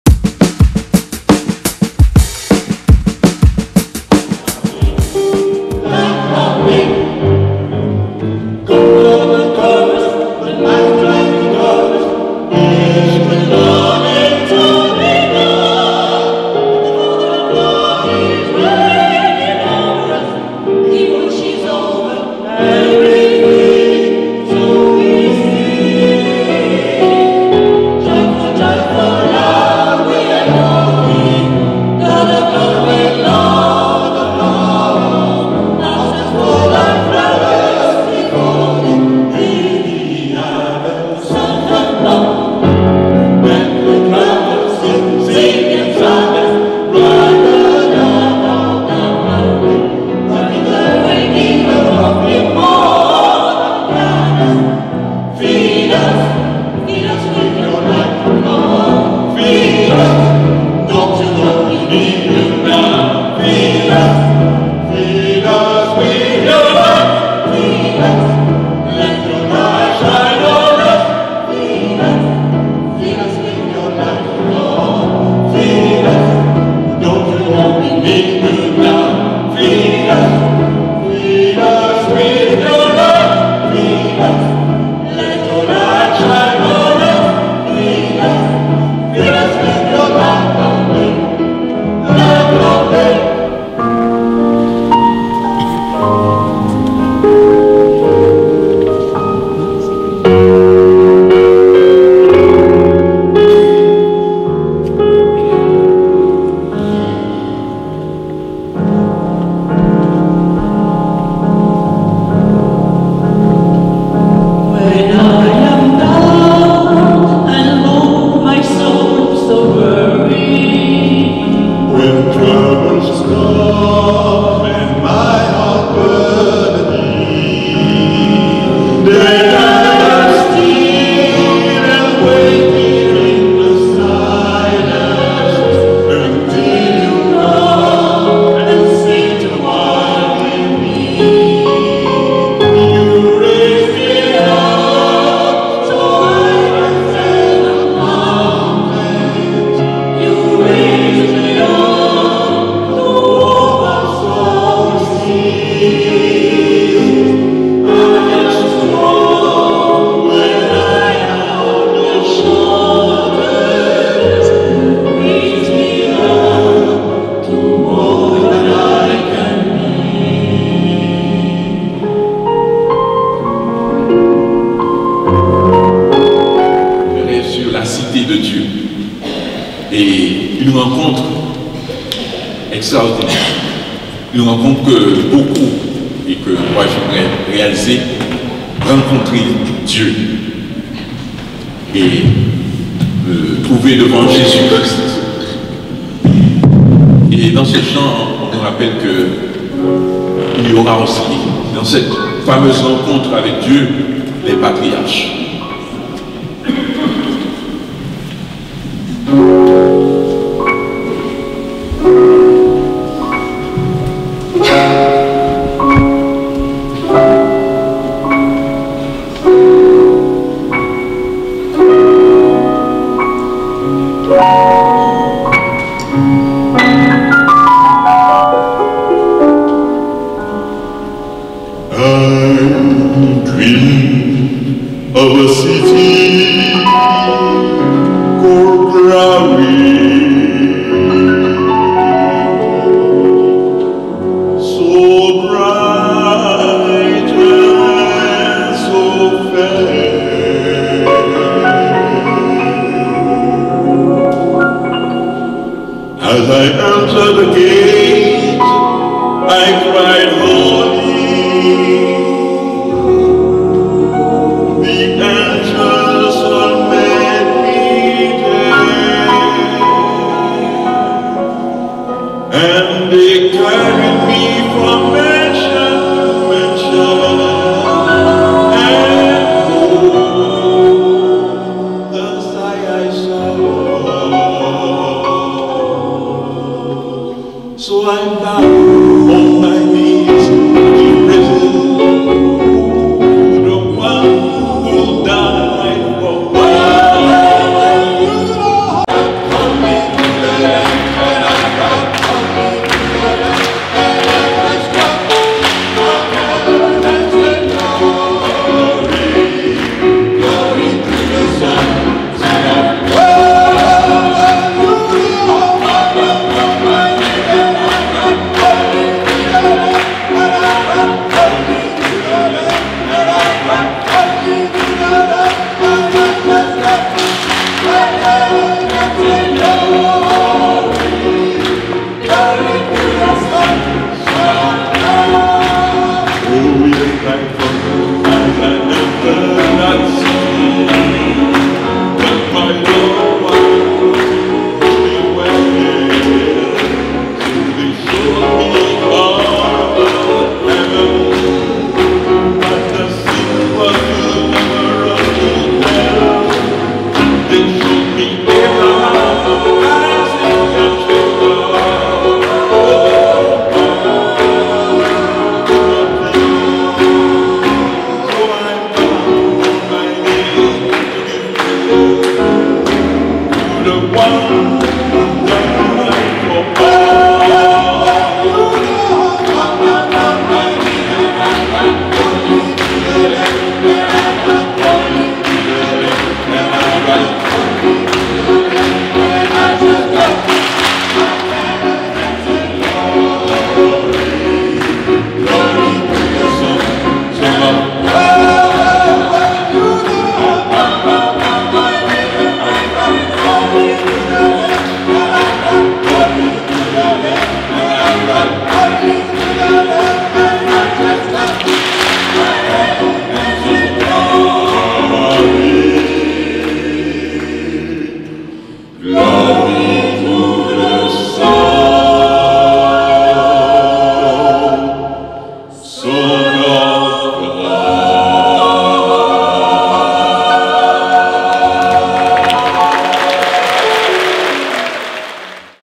PSALMODY QUARTET - PART 2
Negro spirituals, interprétés par le "PSALMODY QUARTET", en l'Eglise Sainte-Croix de Bray-sur-Seine, samedi 12 octobre 2024.